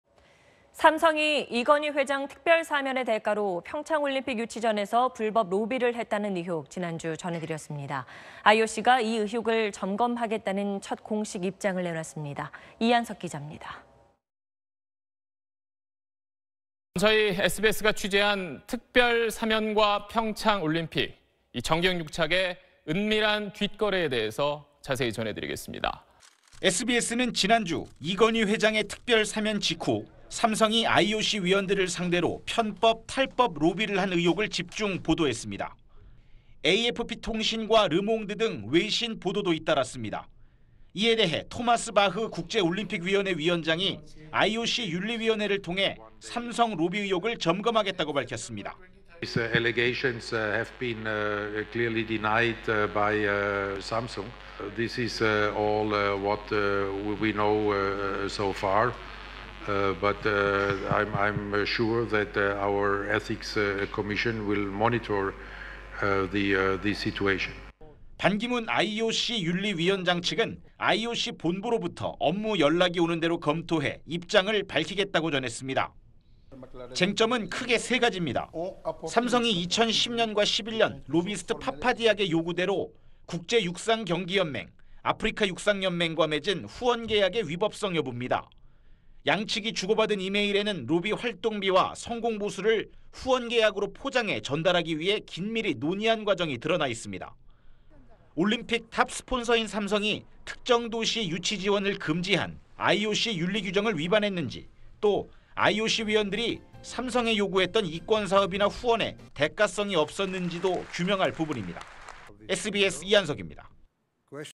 IOC "삼성 '평창 불법로비 의혹' 점검"…3가지 쟁점은? / SBS
삼성이 이건희 회장 특별사면의 대가로 평창올림픽 유치전에서 불법 로비를 했다는 의혹을 지난주 전해드렸습니다. IOC가 이 의혹을 점검하겠다는 첫 공식입장을 내놨습니다.

이한석 기자입니다.